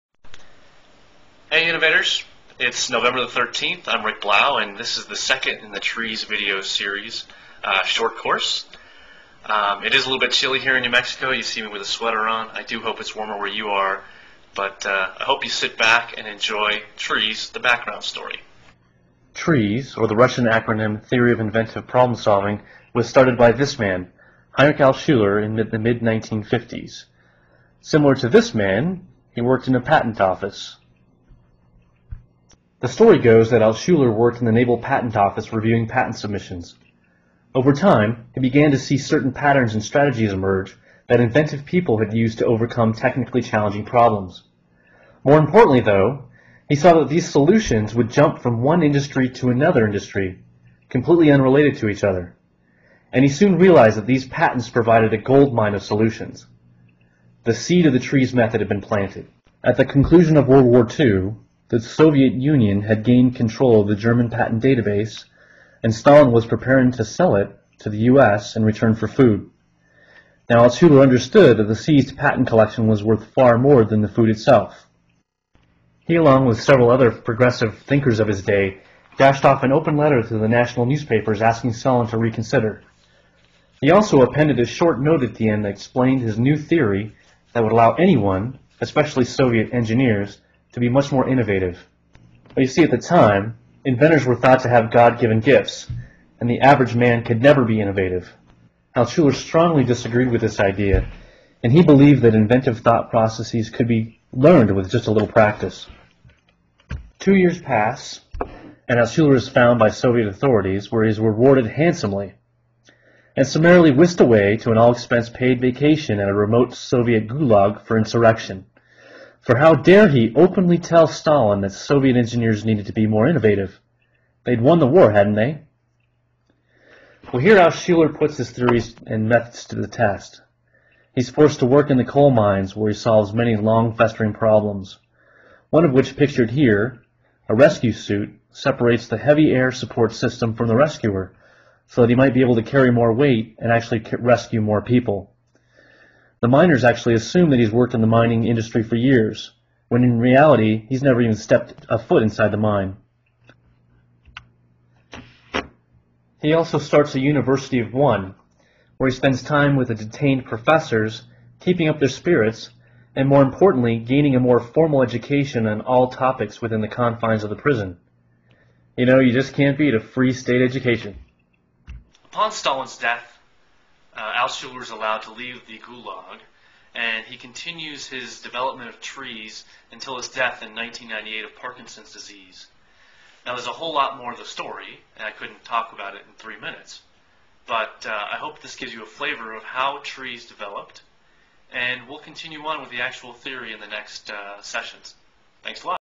TRIZ shortcourse #1-- TRIZ backstory
The first in the series-- Where did TRIZ come from.

idea that engineering systems can be deconstructed into the functional components.